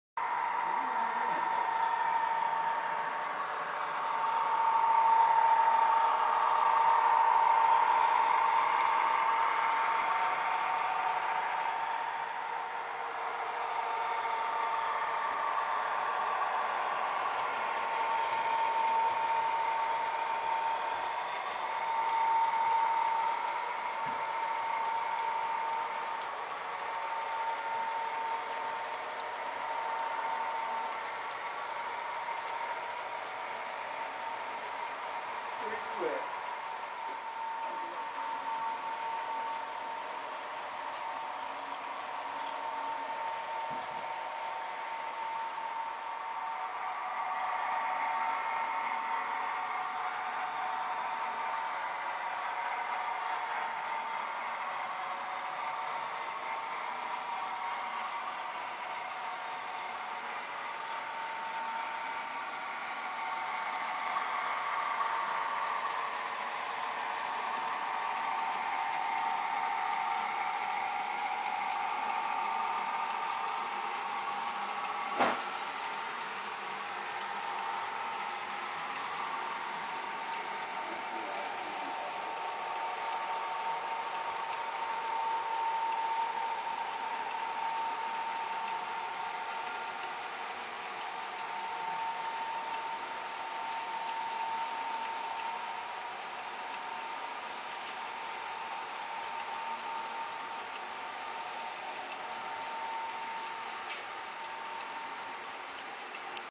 Conrail coal drag.wmv
Video of some recently completed sections of the layout.   This is video of some bridges that were added.